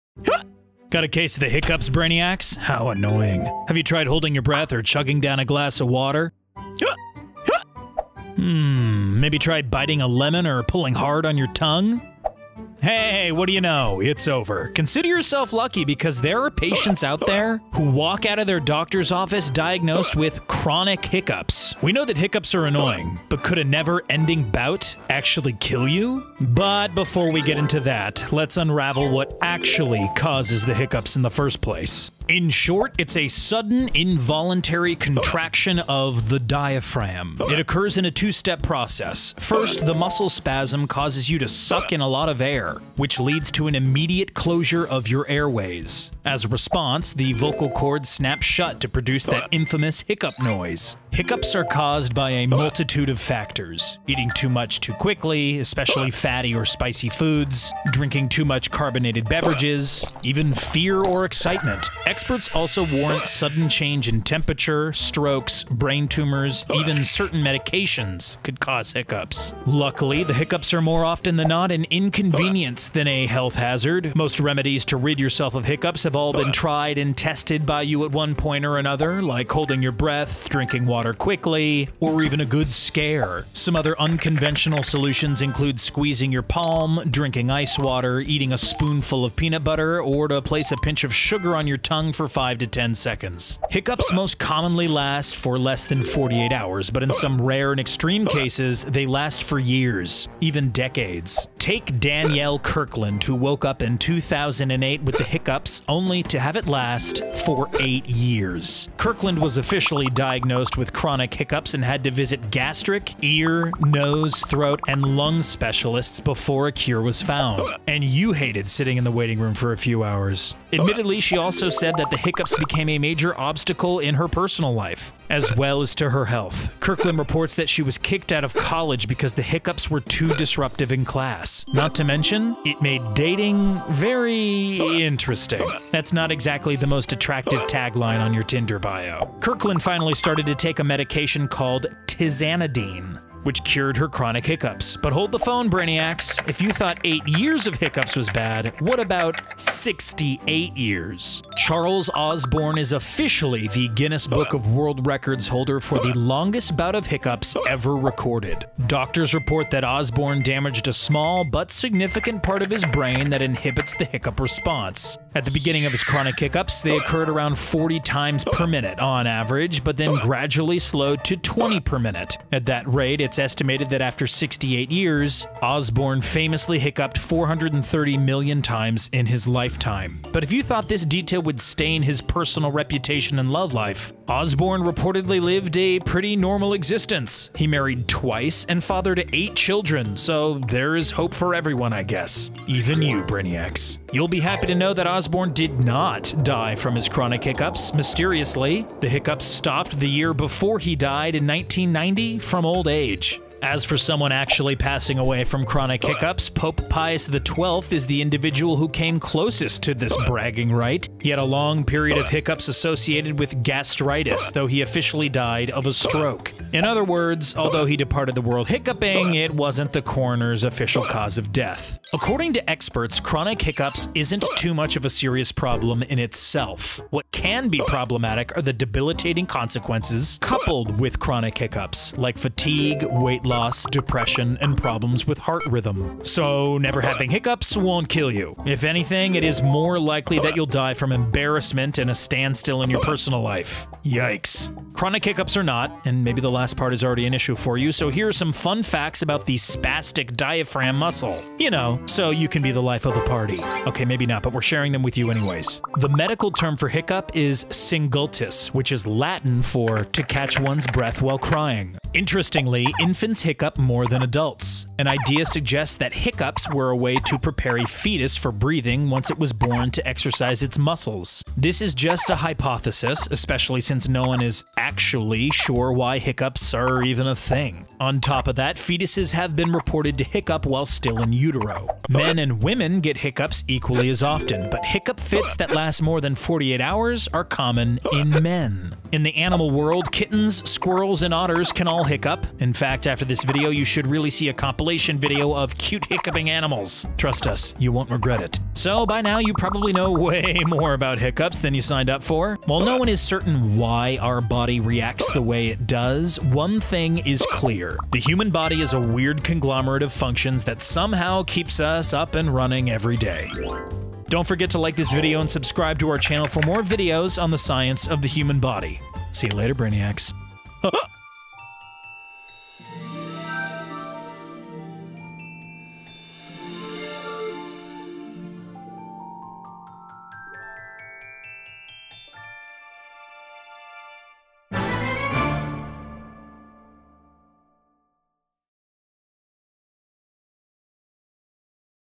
You Can Be Stuck With Hiccups For The Rest of Your Life
What Causes Hiccups? Now Imagine If Could Never Stop Your Hiccups.

Do you ever find yourself not properly chewing and then swallowing a big slice of pizza? What follows? Hiccups of course! Sure, it’s comical for a short while, but THEN it gets annoying. Before you know it, your internet search history revolves solely around the different methods of getting rid of hiccups. This is UNBEARABLE. And just as you start to lose all of your patience, they go away. Phew, what a relief. Could you imagine what it would be like if you had hiccups for 68 YEARS STRAIGHT? Also known as chronic hiccups, for some unknown reason, some patients walk out of the hospital doors with an UNCONTROLLABLE fit of hiccups. Doctors and medical experts can’t always explain it, but surely, there must be a reason?

In this video, we investigate chronic hiccups, as well as their causes. Plus, we offer you some solutions for you to try over your next hiccup fit.

Did someone say hiccup-based fun facts? We know, you didn’t ask, but we’ve got them for you anyways, Brainiacs. Then, later in the video, you meet Charles Osborne and Danielle Kirkland who couldn’t stop hiccuping for a combined 76 years. Their lives must have been pretty miserable, right? Not exactly a quality you would include on your Tinder profile….but, you’d be surprised how normal their lives ended up despite their *special* medical condition. All of that, and more in this video, Brainiacs. You don’t want to miss this one!